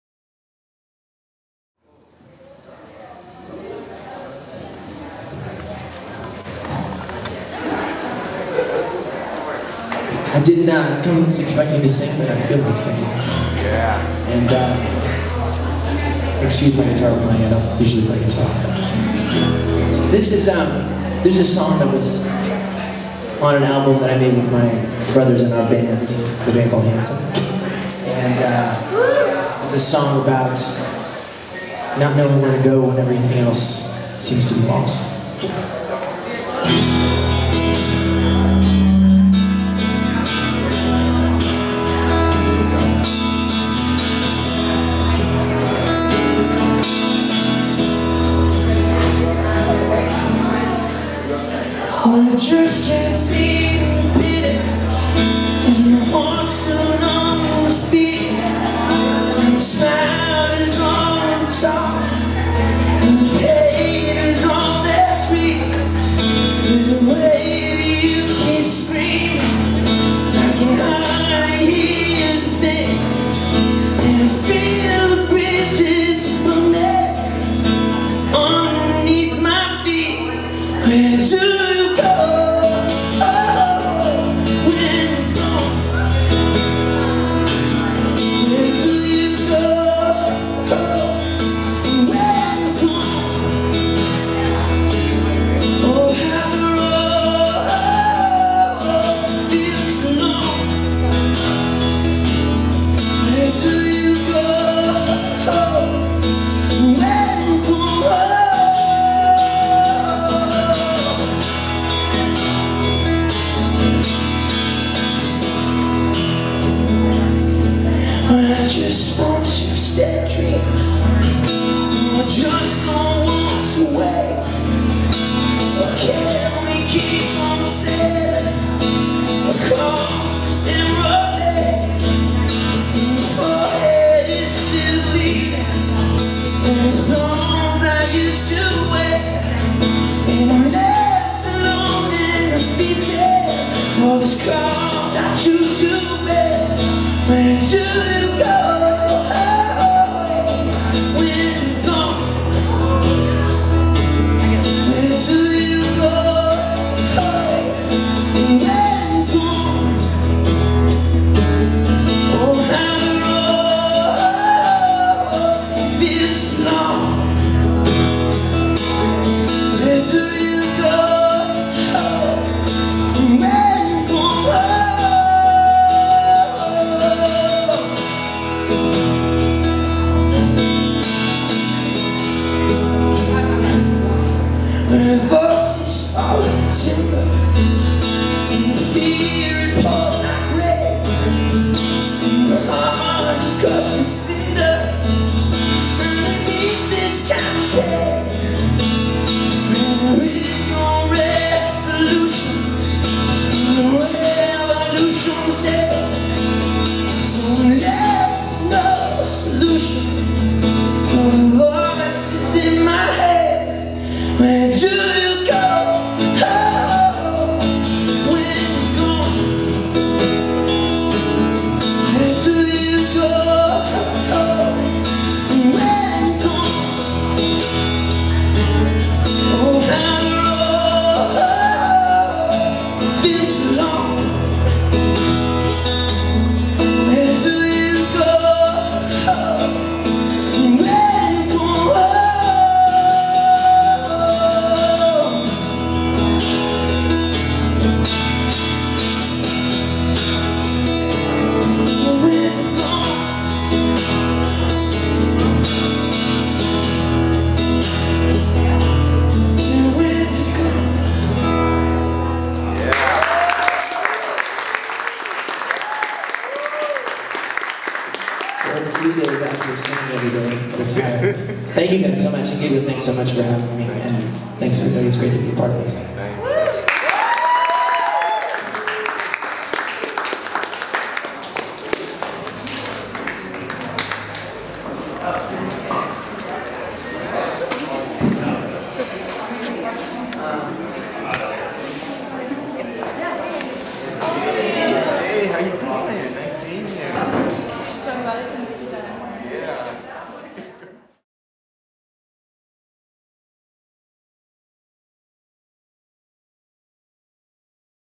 Taylor Hanson performs at the My Home Darfur exhibit opening
Eco-LogicalART
Los Angeles

Please sign our petition - A Peaceful HOME for the Children of Darfur